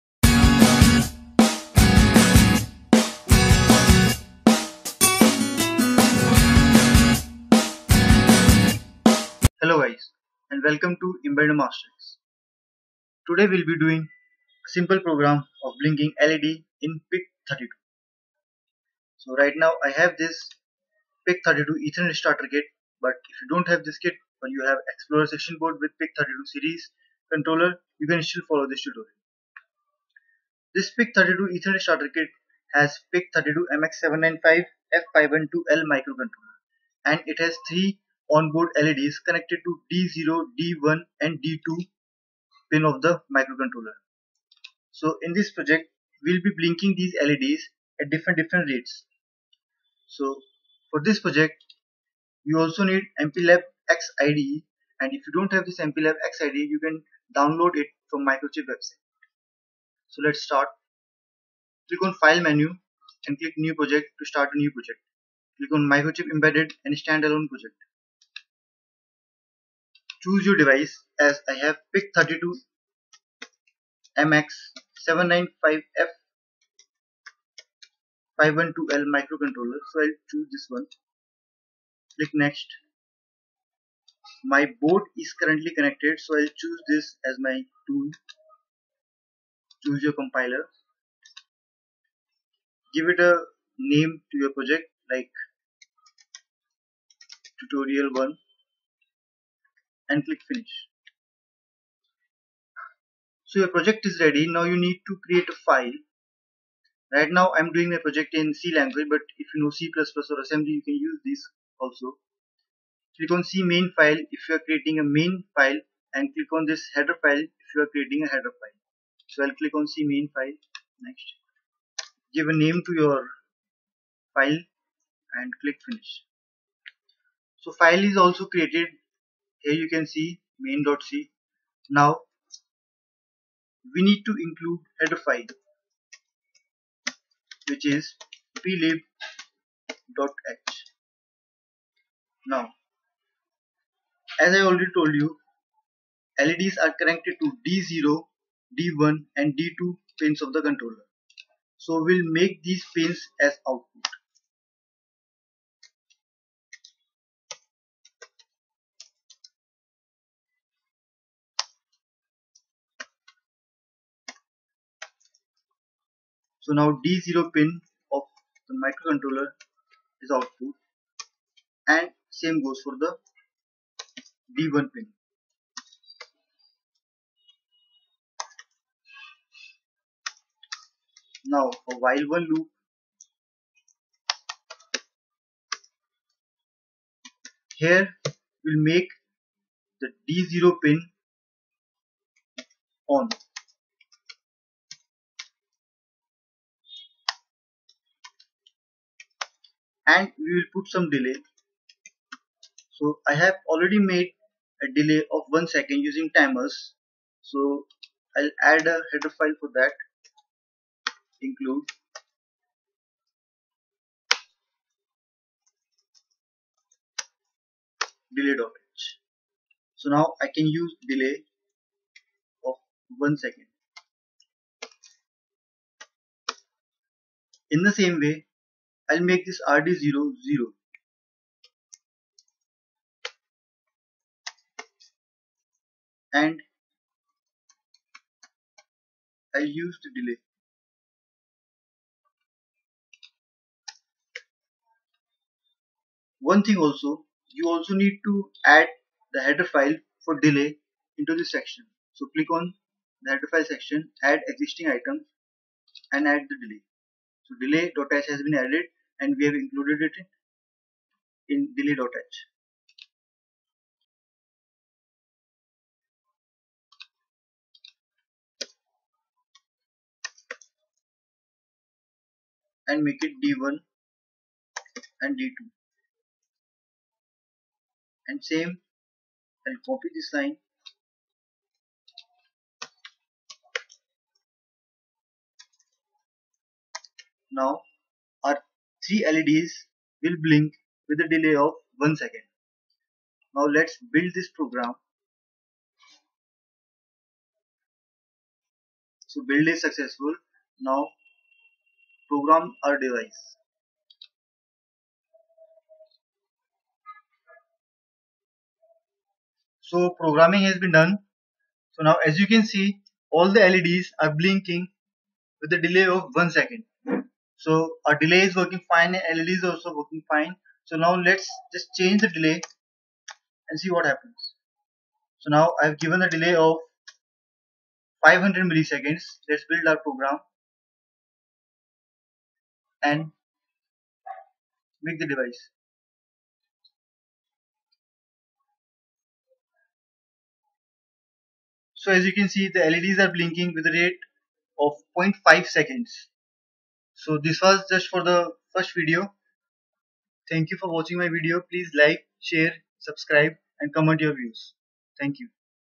PIC32 Tutorial Series | Getting Started & Blinking Led
In this tutorial, we will show you how to get started in PIC32 microcontroller and blink an led.